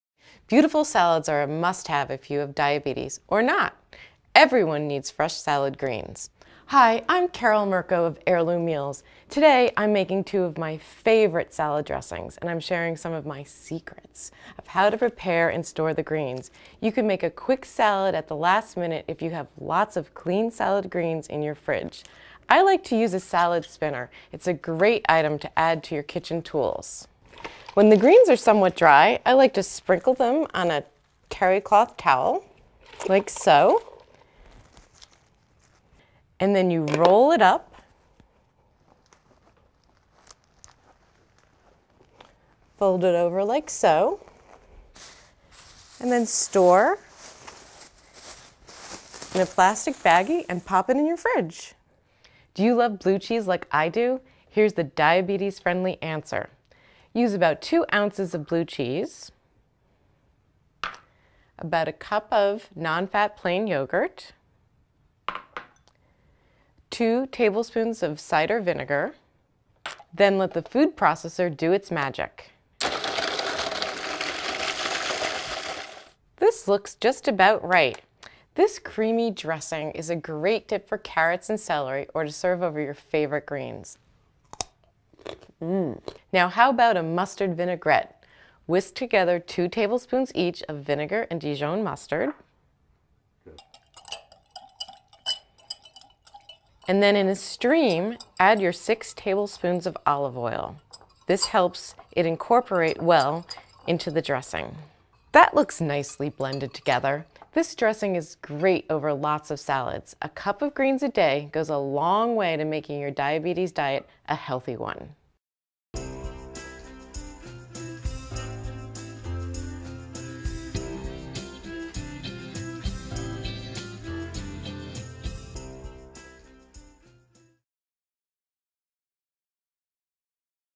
Diabetes Diet: Blue Cheese and Mustard Vinaigrette Dressing Recipes
Beautiful salads are a must-have if you have diabetes, or not! Here are two recipes for salad dressings that are great for managing diabetes and your diabetes diet.

If you need help with diabetes management or diabetes supplies call a Liberty Medical representative at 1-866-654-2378 today.